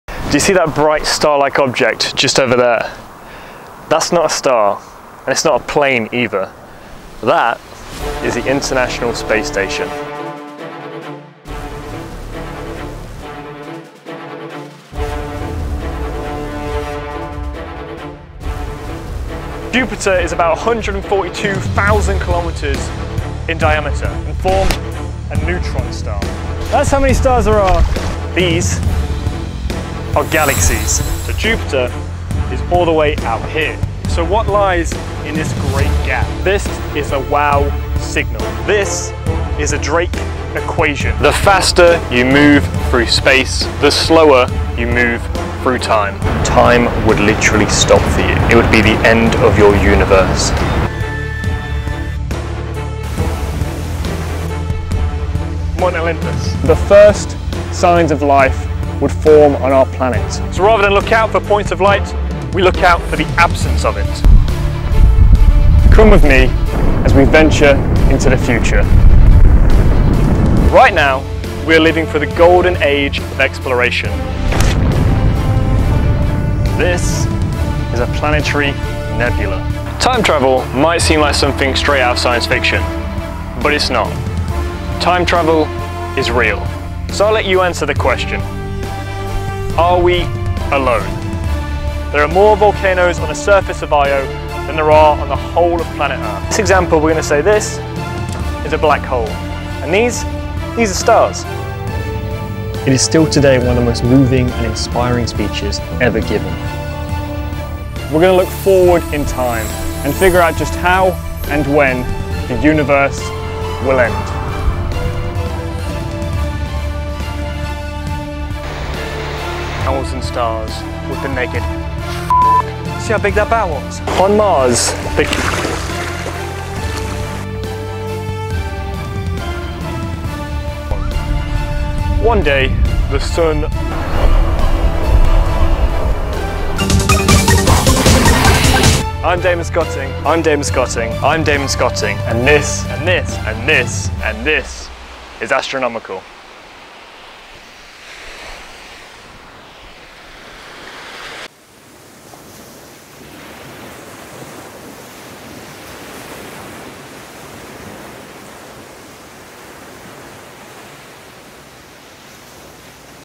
𝐀𝐒𝐓𝐑𝐎𝐍𝐎𝐌𝐈𝐂𝐀𝐋 𝐒𝐄𝐑𝐈𝐄𝐒 𝟏 𝐓𝐑𝐀𝐈𝐋𝐄𝐑 👨‍🚀✨🚀(4K)
Hello,
This is Astronomical!🔭🚀✨
A space documentary series that I have been working on for the past few years.
In it, I answer some of the universes biggest questions such as: Where do we come from, Are we alone and How will the universe end? 🪐⚛💫

Over the course of 7 Jam packed episodes we'll discover some of the universe's biggest secrets and gain a deeper understanding about our true origins and what lies in store for us next, in this cosmic voyage. 🌌🌟👨‍🚀

Series 1 episode list:

EP 1: We are stardust (May 1st) ⭐
EP 2: Everything we see is in the past (May 15th) ⭐
EP 3: Are we alone? (May 31st) ⭐
EP 4: The 7 wonders of the Solar system (June 12th) ⭐
EP 5: The 8 wonders of the Universe (June 26th) ⭐
EP 6: We are explorers (July 10th) ⭐
EP 7: How the Universe will end (July 24th) ⭐

As well a 3-part behind the scenes look at what it took to make the series, including how I managed to travel, write, shoot and edit the entire documentary series on my own, whilst studying for a degree in Astrophysics and working full time at the National space centre!

If you don't have a keen interest in Space or Astronomy now, then I guarantee you will after watching this series.

It's going to be a lot of fun and very educational!

✨THIS IS ASTRONOMICAL ✨